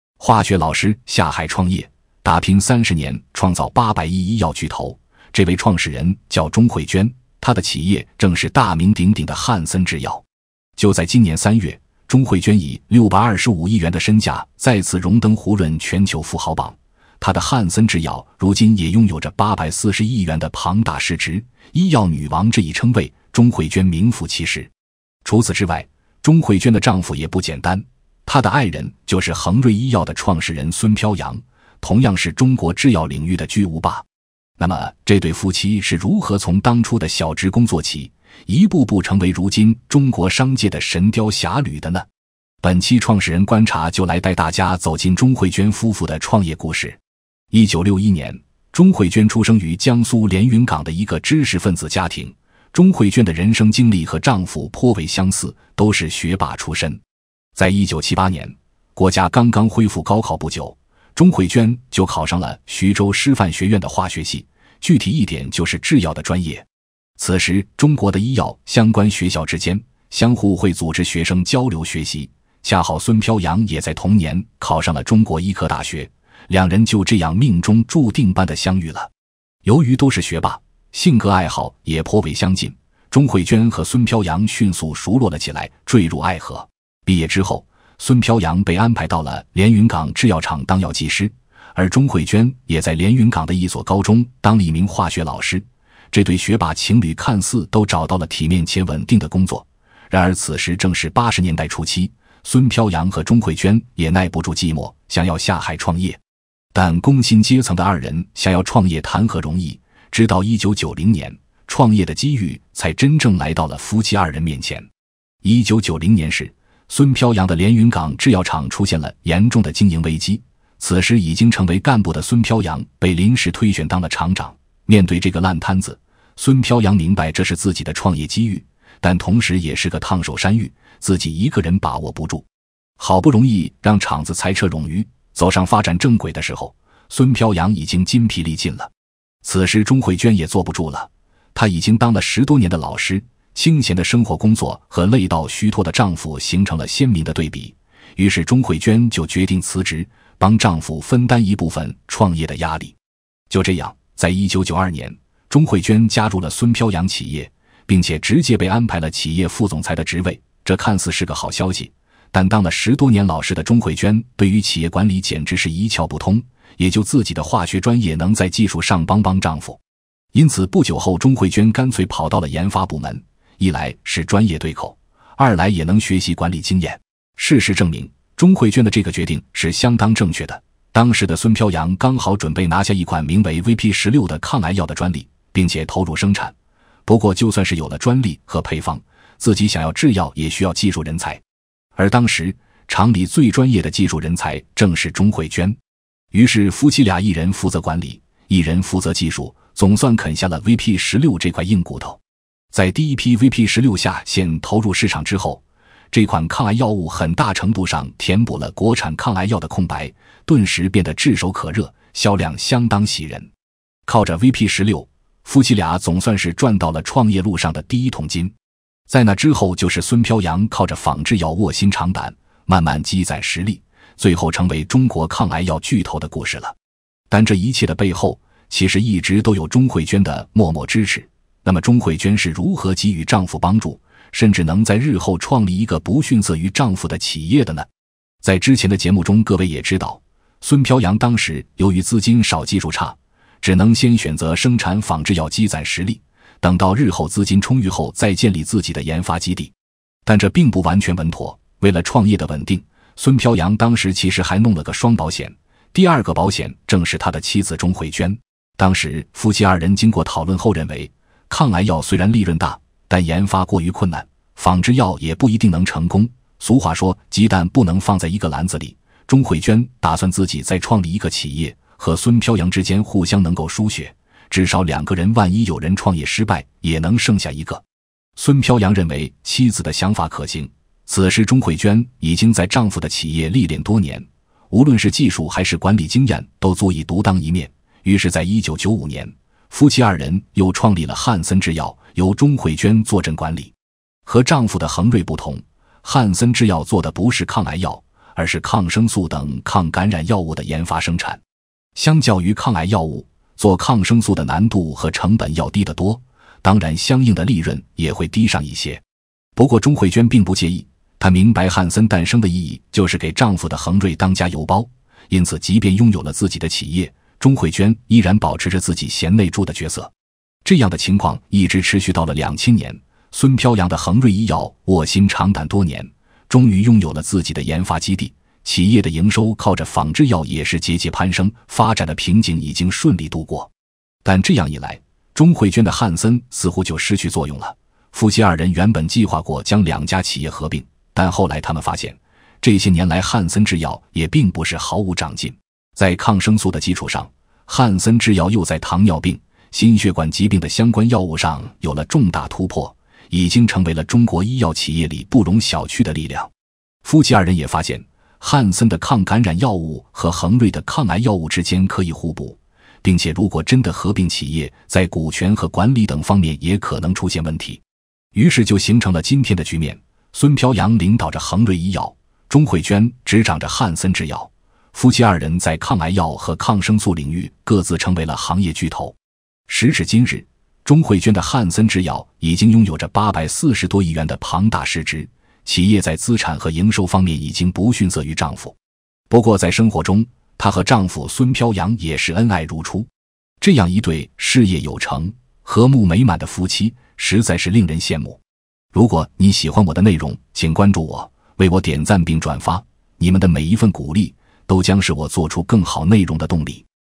“医药女王”钟慧娟，化学老师逆袭富豪，白手起家创业赚上千亿！
化学老师下海创业，打拼30年创造800亿医药巨头，这位创始人叫钟慧娟，她的企业正是大名鼎鼎的翰森制药。就在今年3月，钟慧娟以625亿元的身价再次荣登胡润全球富豪榜，她的翰森制药如今也拥有着841亿元的庞大市值，医药女王这一称谓，钟慧娟名副其实。除此之外，钟慧娟的丈夫也不简单，她的爱人就是恒瑞医药的创始人孙飘扬，同样是中国制药领域的巨无霸。那么这对夫妻是如何从当初的小职工做起，一步步成为如今中国商界的神雕侠侣的呢？本期创始人观察就来带大家走进钟慧娟夫妇的创业故事。